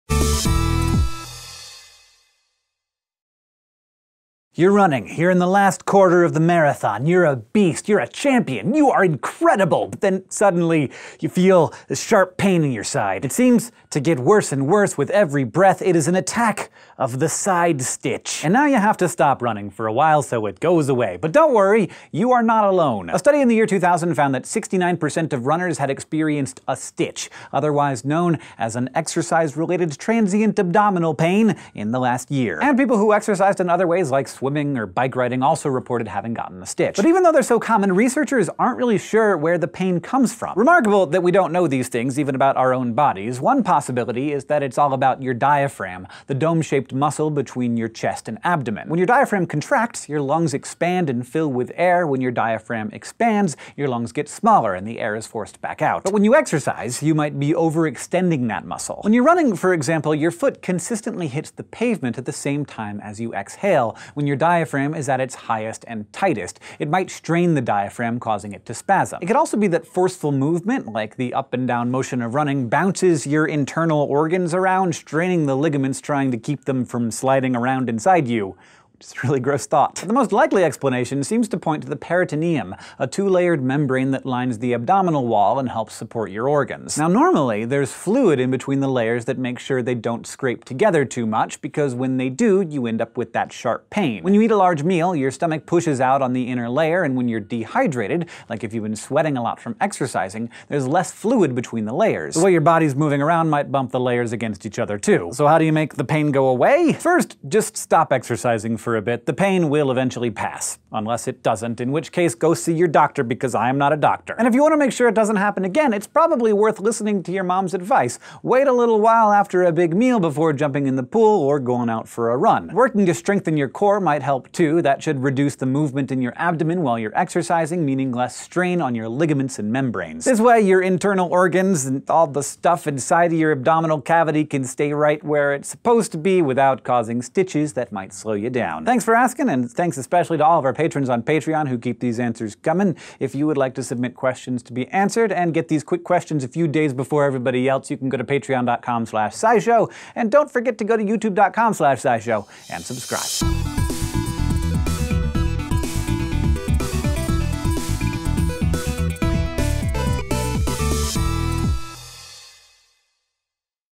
What's Causing That Stitch in Your Side?
What's the deal with that sharp pain in your side when you're trying to win that marathon? SciShow has the answers!